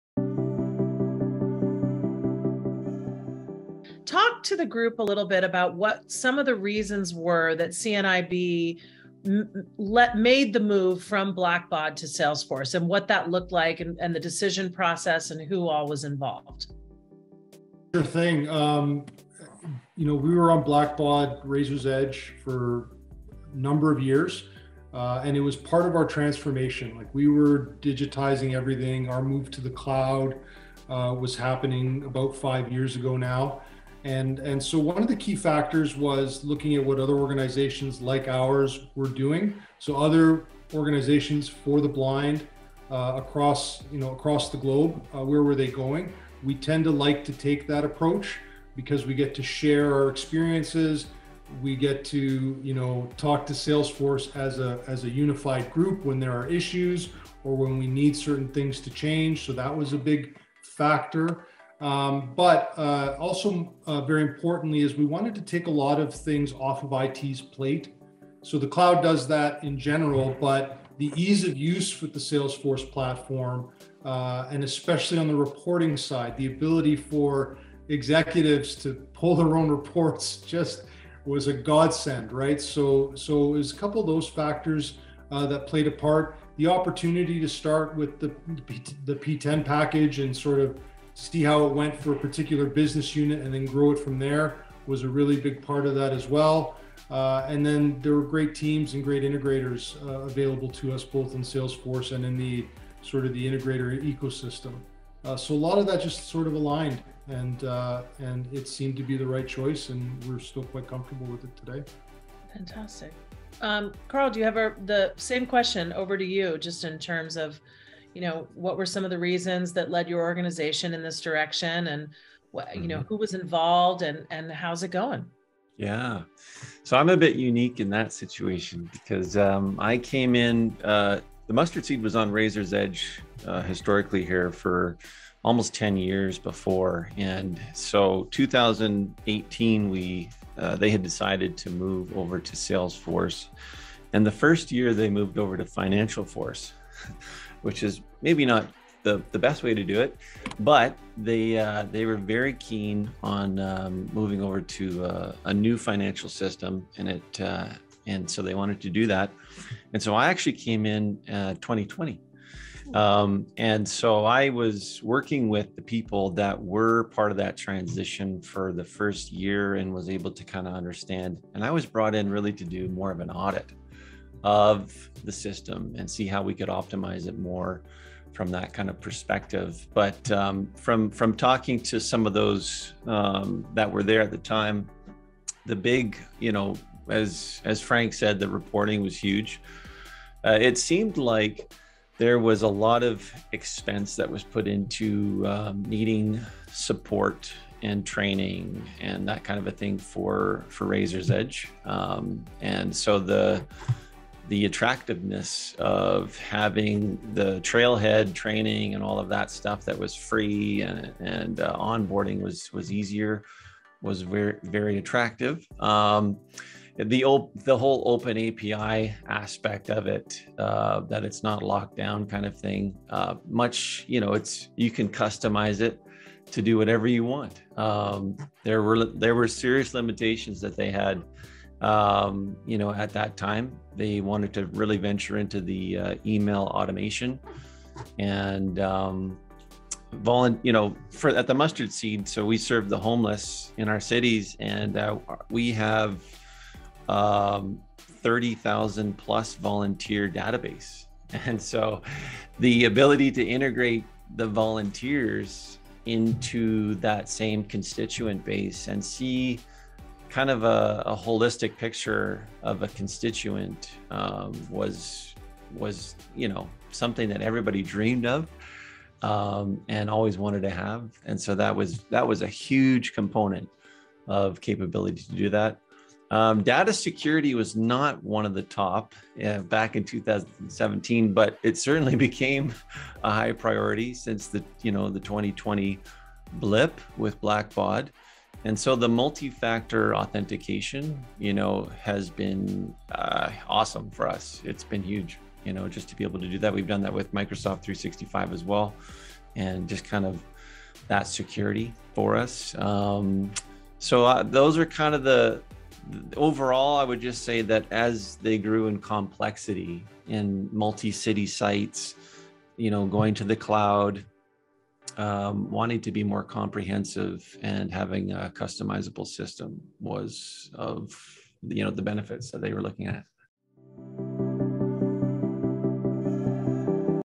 Webinar Shorts #1: Why Switch from Blackbaud to Salesforce?
Watch a snippet of our Blackbaud to Salesforce, Eh? webinar hosted by Sterrin Bird (Salesforce, Nonprofit Advisor) to hear directly from our speakers Karl Feltmate (Senior Manager, Stewardship and Donor Retention at The Mustard Seed) and Frank Lombardo (Vice President of IT and Digital Ecosystems at Canadian National Institute for the Blind) about their experience transitioning their organizations from Blackbaud to Salesforce.

Watch the full video linked at the end for in-depth and transparent discussion about the journey from Blackbaud to Salesforce from 2 organizations (The Mustard Seed & CNIB) who've been there and done it!